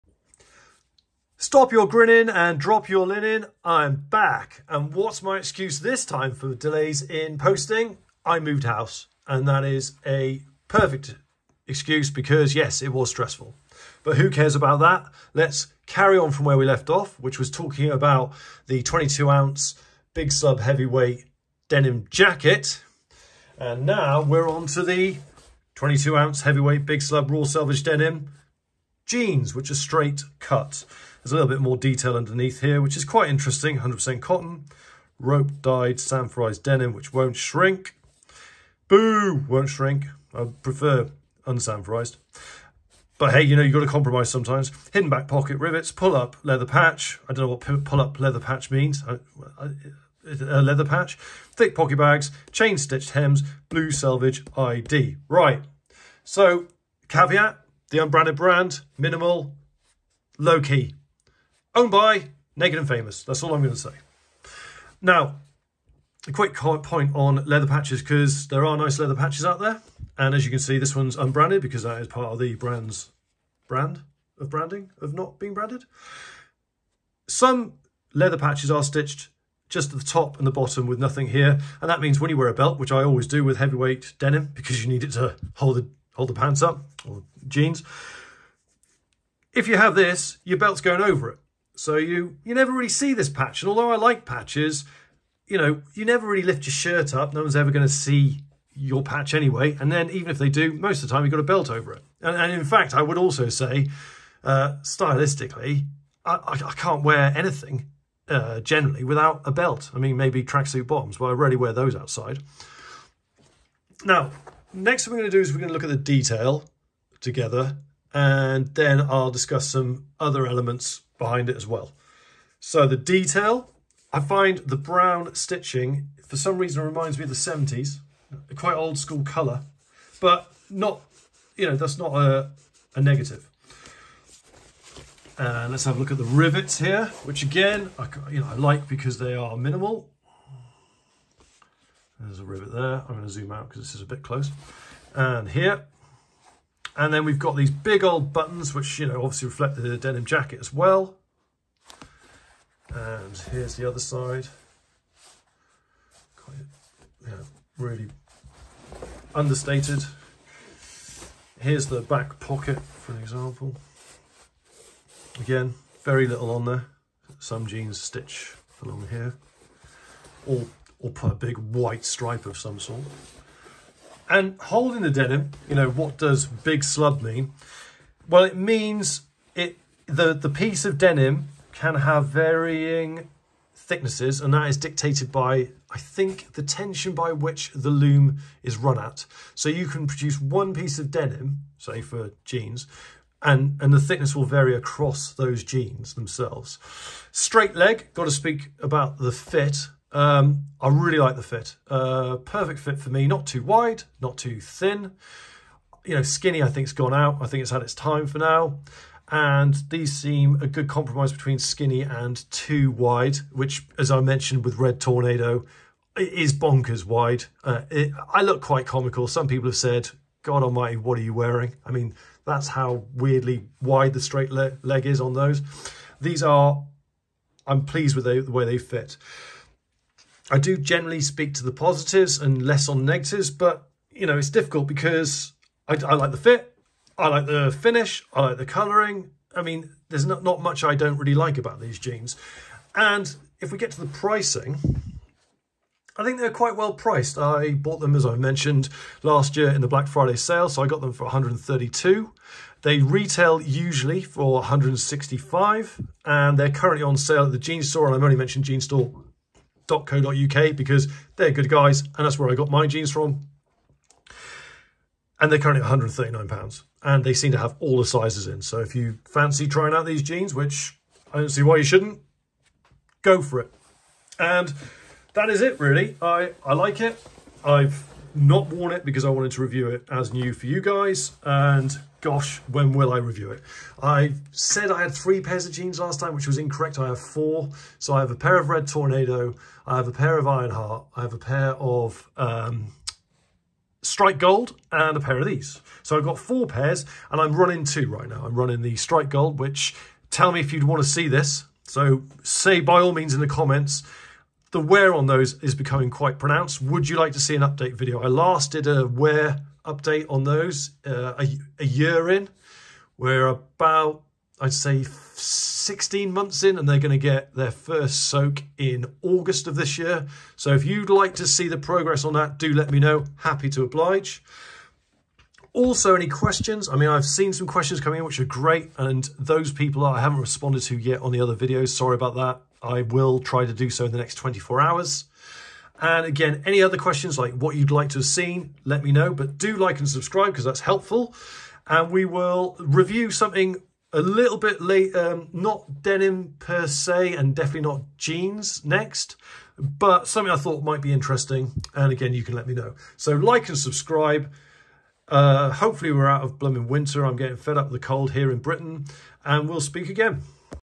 The unbranded brand - 22oz Heavyweight Big Slub Raw Selvedge Denim - rope dyed, sanforized
Today I review the Unbranded brands 22oz big slub.

You currently currently purchase these jeans on discount at Jeanstore